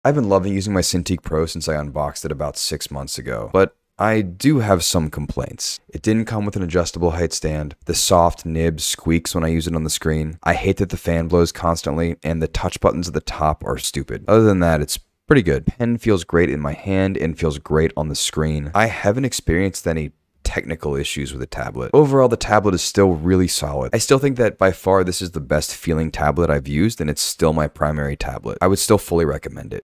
Cintiq Pro 6 months later...
Re-reviewing the Cintiq Pro 24 I bought 6 months ago. I still love it, but have some complaints.